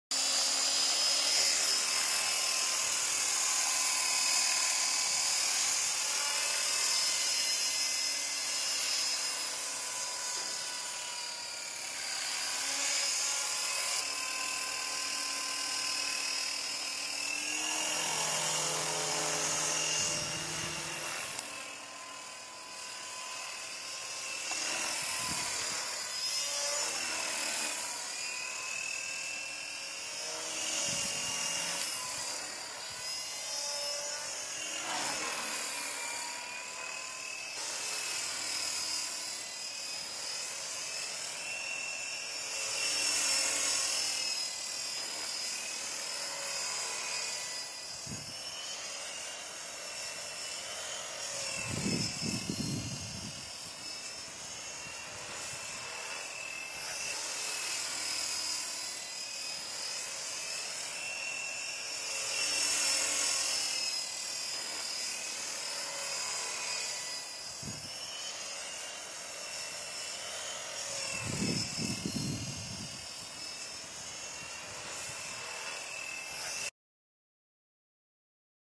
Falcon 400 CCPM Art-tech chez Miniplanes
le dernier né de la fantastique série de Falcon 400.
Avec plus de 1000 modèles vendus le Falcon 400 est le plus grand succès hélico chez Miniplanes.
Le dernier né est donc né sous un bon signe, le Falcon 400 CCPM est étonnant, malgré sa petite taille il autorise toute la voltige helico RC